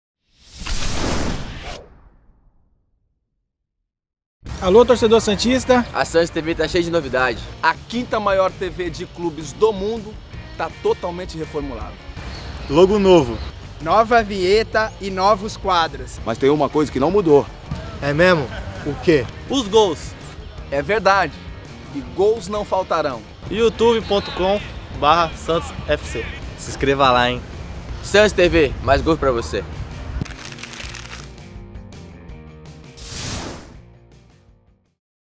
Nova identidade visual da Santos TV
A Santos TV está de cara nova!! Novo logo, novas vinhetas e novos quadros!!